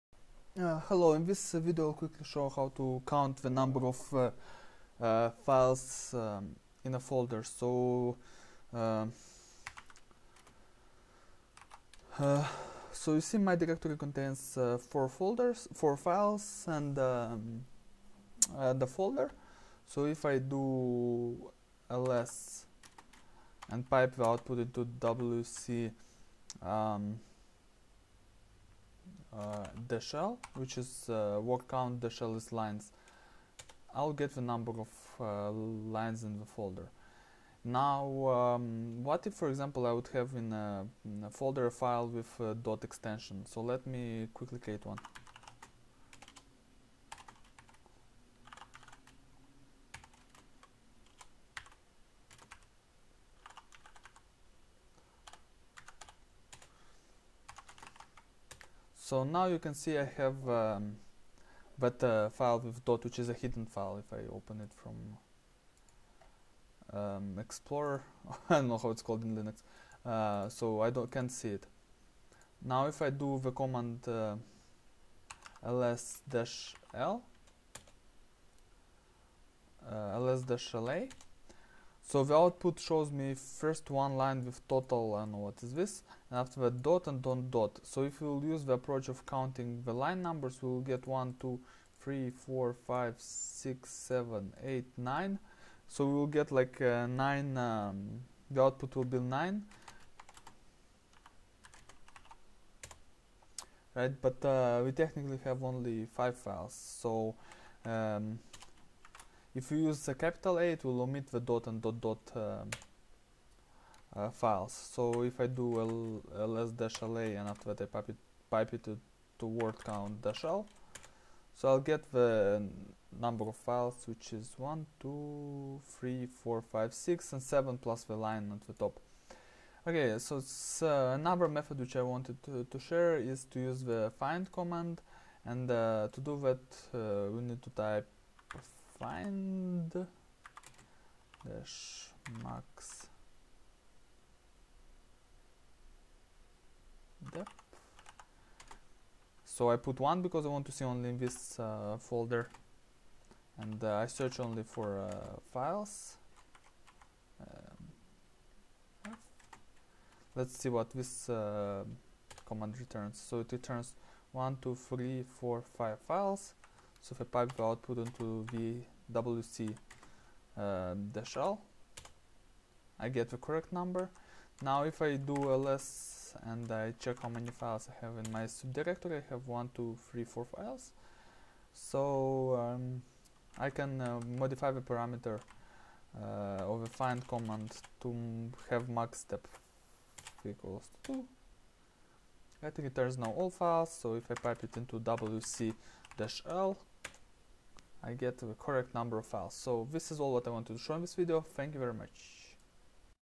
Unix/Linux Count Number of Files In Folder
This video demonstrates Unix/Linux commands to count the number of files in the directory

1) ls -l | wc -l
2) find -maxdepth 1 -type f | wc -l